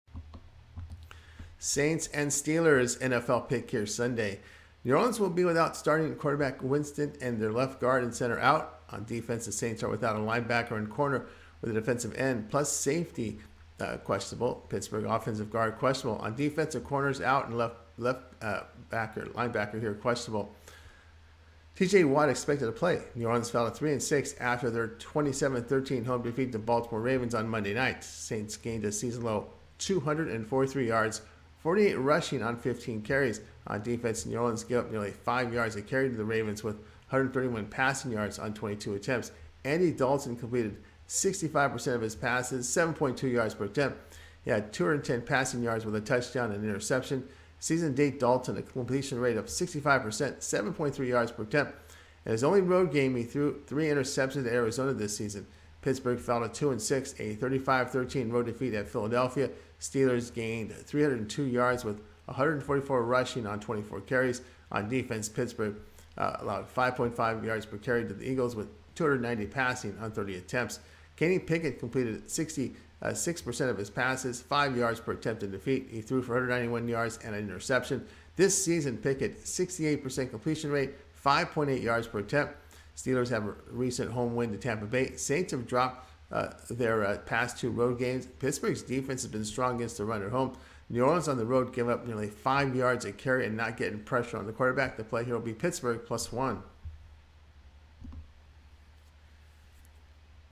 New Orleans Saints vs. Pittsburgh Steelers 11/13/2022 Week 10 FREE NFL Picks on NFL Betting Tips
11/13/2022 New Orleans Saints vs. Pittsburgh Steelers FREE NFL Picks and Predictions on NFL Betting Tips for Today